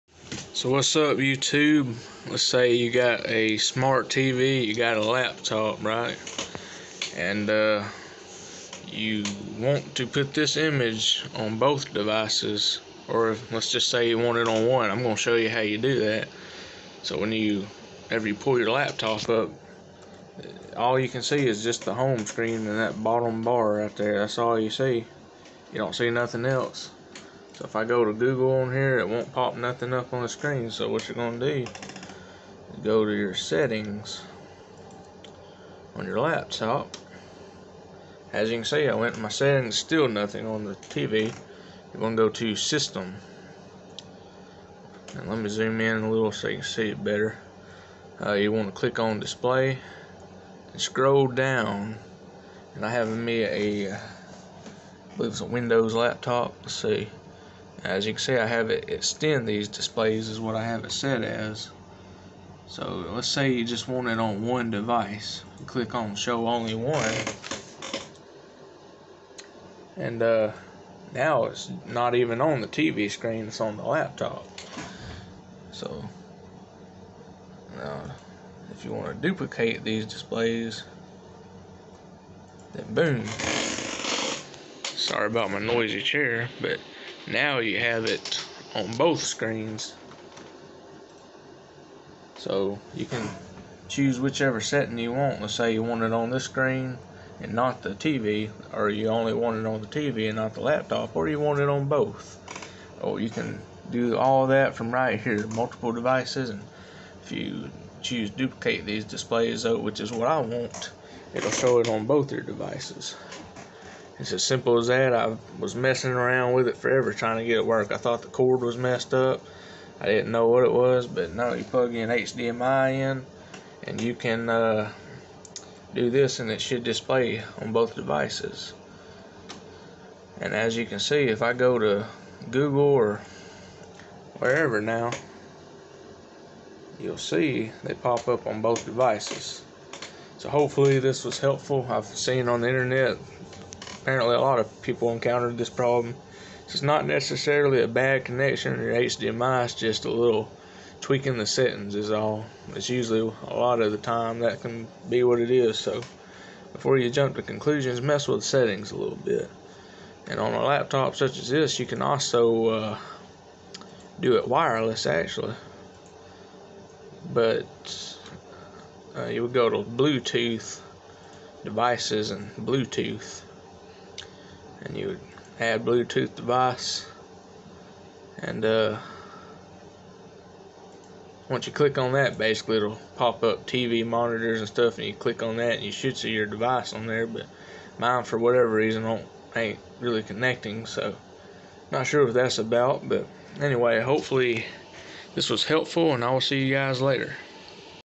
Laptop Not Displaying on TV or Only Shows Background/Desktop Fix
In this video I show you how to fix a laptop that won't display on your Tv or will only stay on the desktop screen and show only the background. This is the fix I used.